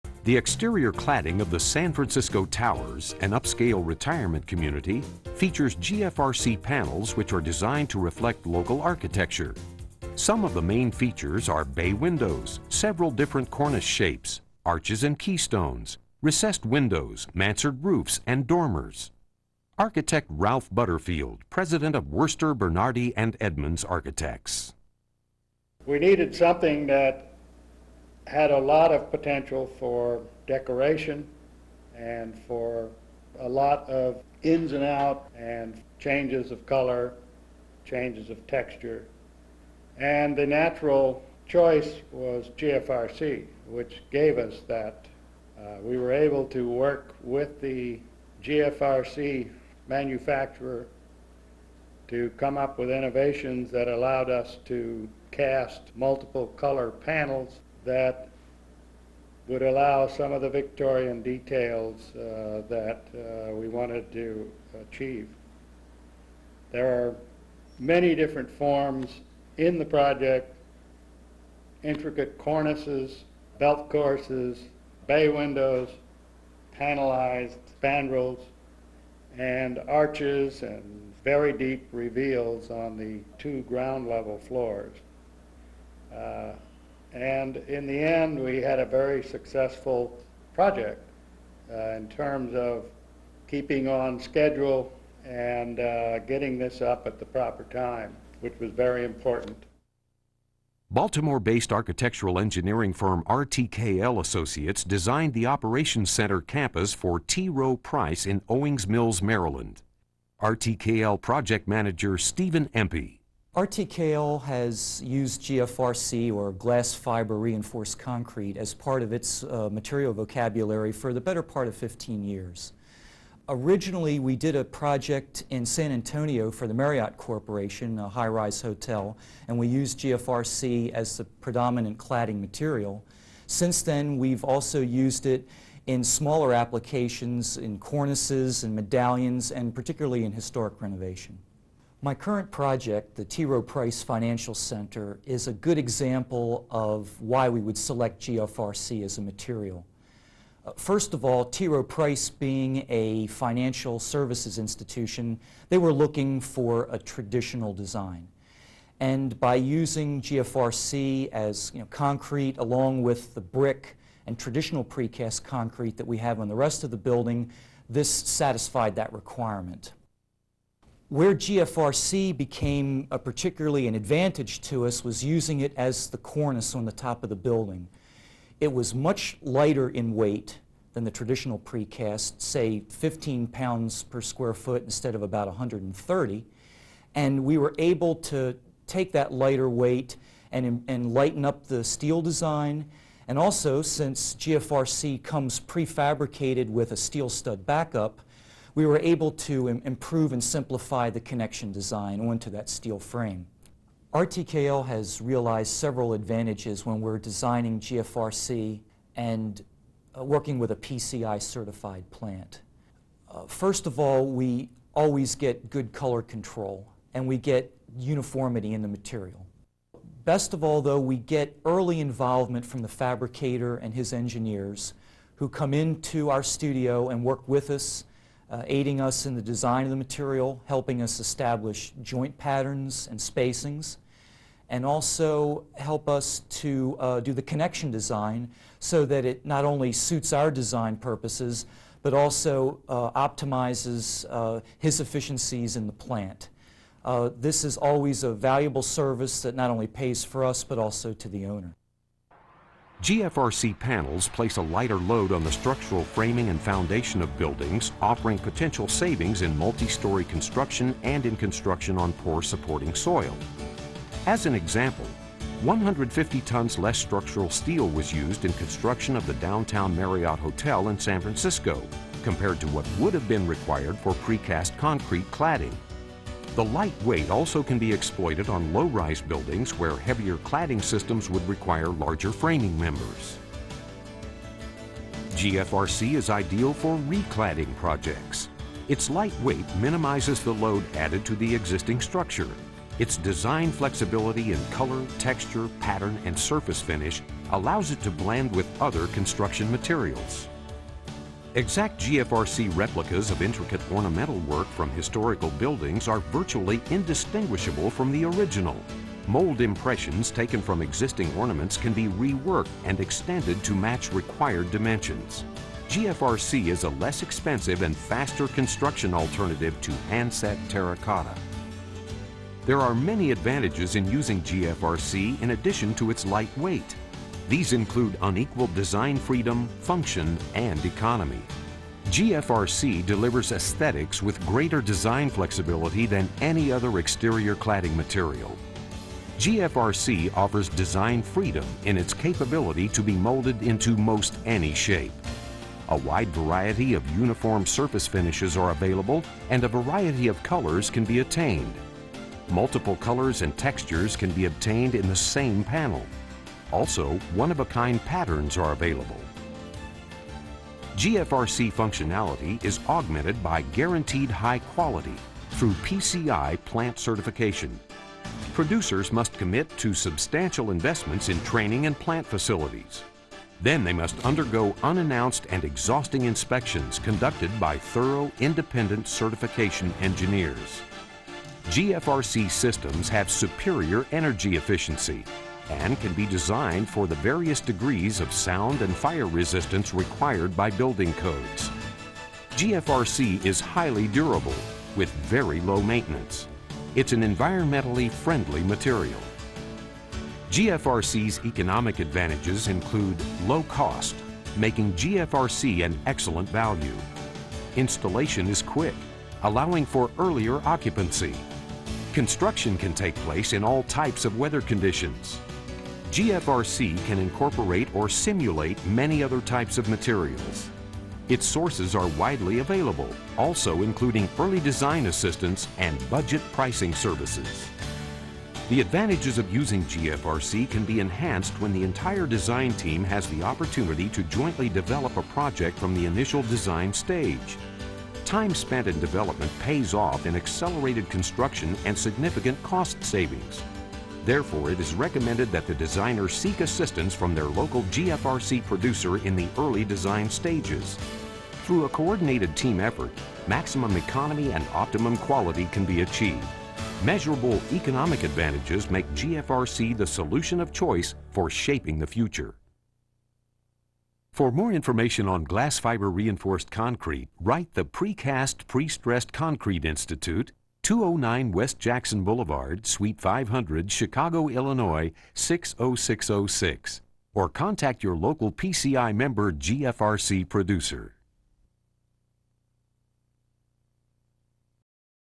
PCI GFRC Part2
Part 2 of a video produced by the Precast / Prestressed Concrete Institute describing Spray-Up GFRC.